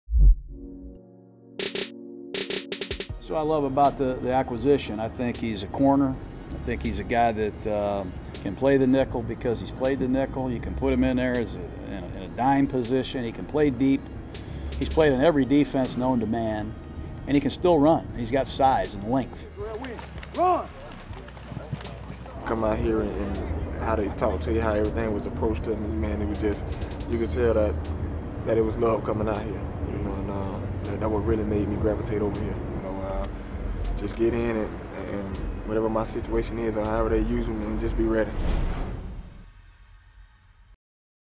Coach Gruden talks Rodgers-Cromartie: "He's got size and length"
Raiders Head Coach Jon Gruden discusses the addition of CB Dominique Rodgers-Cromartie on Monday.